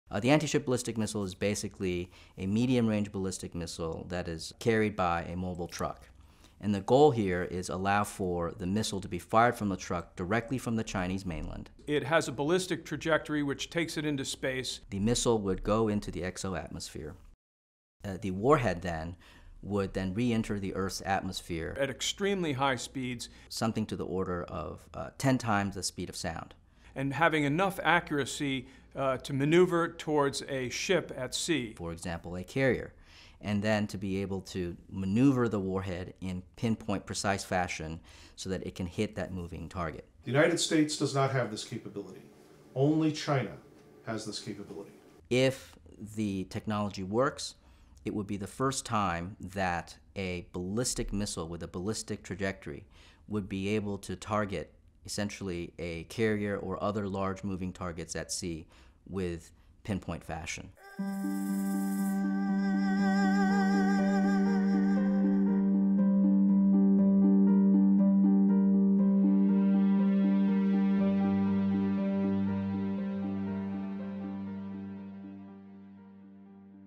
Professor Yoshihara Talks Anti-Ship Ballistic Missile
Naval War College’s Toshi Yoshihara explains how Chinese want to use their anti-ship ballistic missile to kill American aircraft carriers.